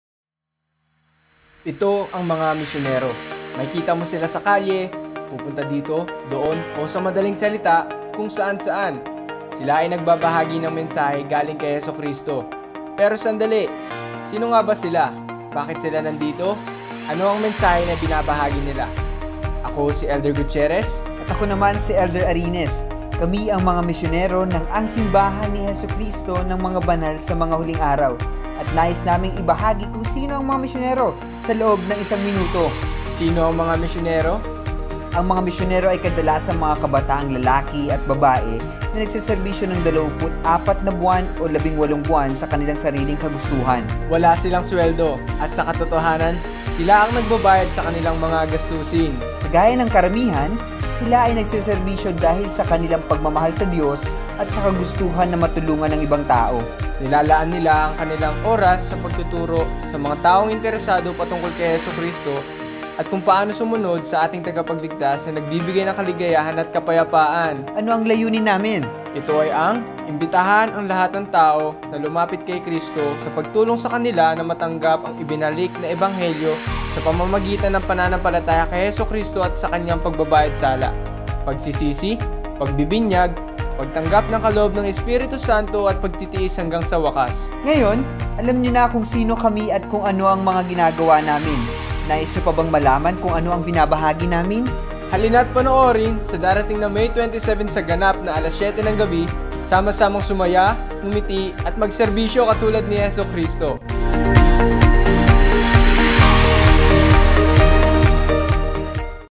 We are the Missionaries
As disciples of Jesus Christ, we share anywhere in the world, whatever your language and wherever you are.  So feel free to join us this coming May 27th in our Missionary take over on the official social media account of The Church of Jesus Christ of Latter-day Saints.